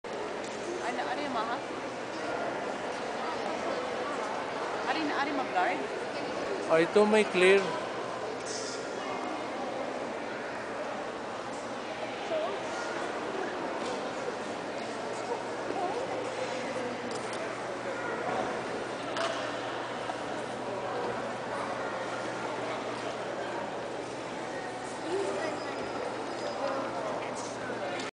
Huge Dinosaur Fossil At the Entrance of the Royal Ontario Museum
A great place to visit when traveling to Toronto is the Royal Ontario Museum in the heart of Toronto, Canada